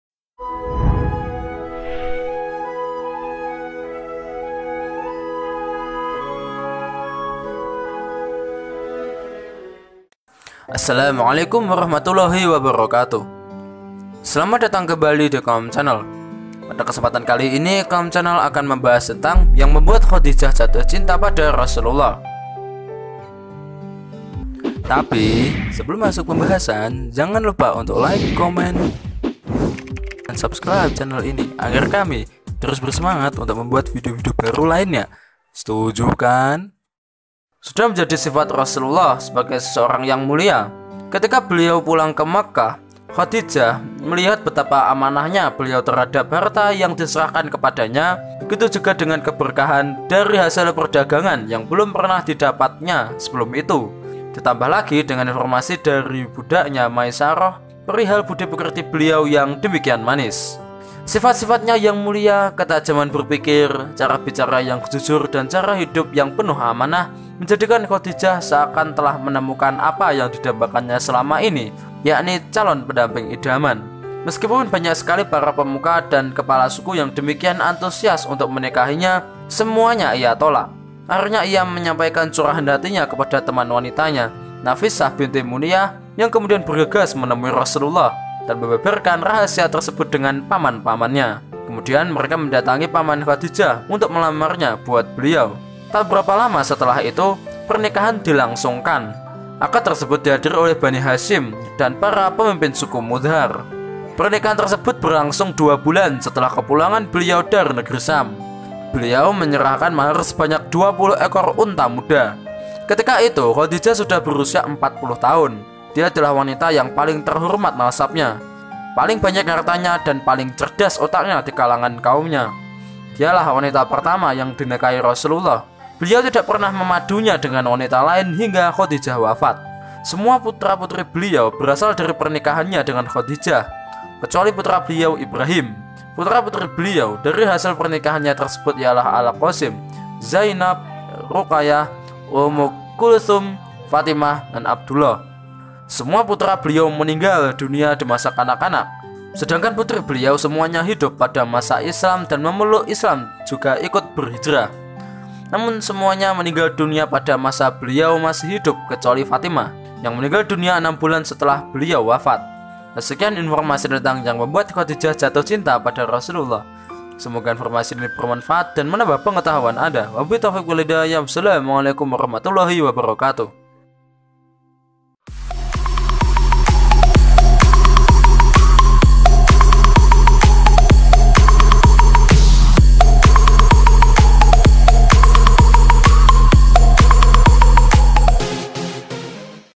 MasyaAllah, Ukhty Harus Tahu !!! Ini Yang Membuat Bunda Khadijah Jatuh Cinta Pada Rasulullah
Berikut ini Informasi seputar islam tentang Yang Membuat Khadijah Jatuh Cinta Pada Rasulullah.
Semoga bermanfaat dan menginspirasi anda.